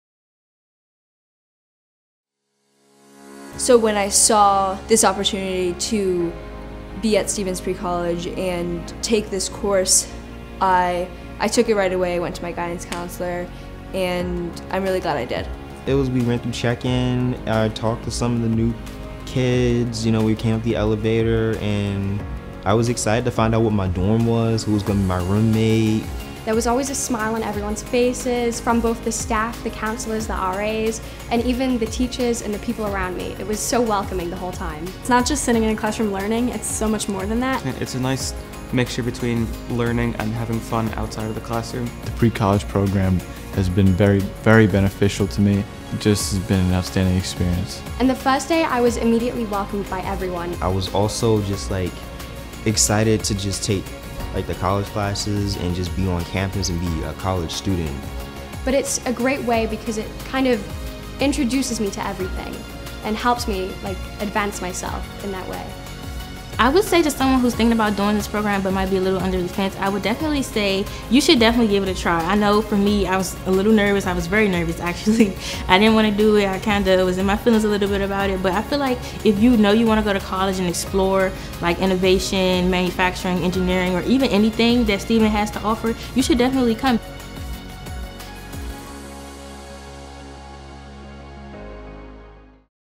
Find Your Future at Stevens
You may not know exactly what you want to do yet, but you know whatever it is, it’s going to be big. When you enroll in a pre-college program at Stevens, you can explore your ideas and interests in a pressure-free, inspiring academic environment that welcomes big thinkers.